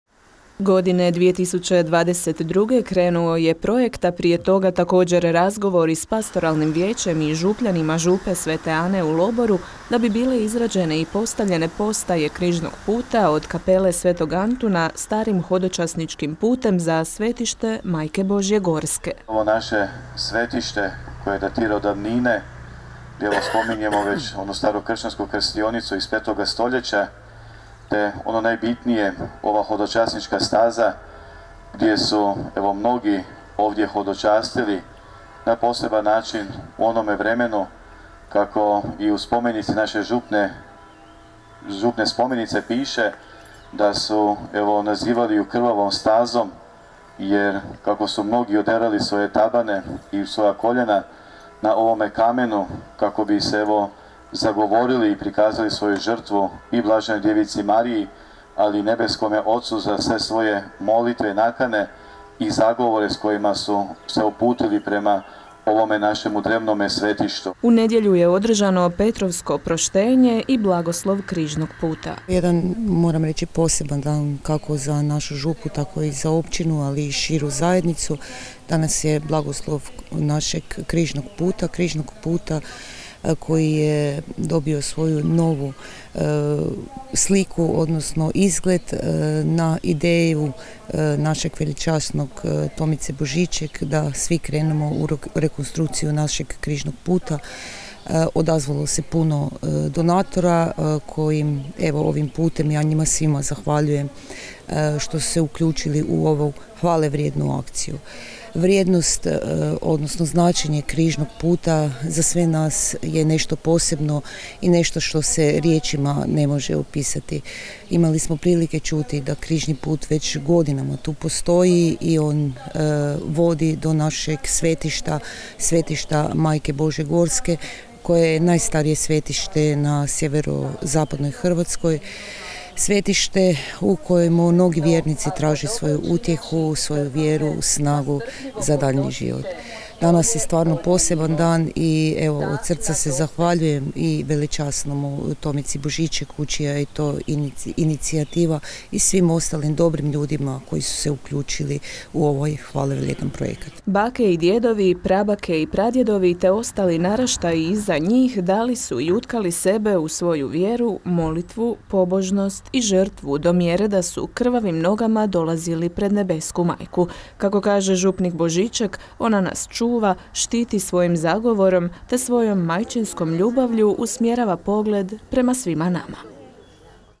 Križni put do svetišta Majke Božje Gorske
Na početku Križnog puta do Svetišta Majke Božje Gorske sve je pozdravio župnik Tomica Božiček. Hodočasnici su potom krenuli stazom, za što im je svima poticaj bio samo se podsjetiti kolika je bila vjera starih koji su pohodili ovo svetište.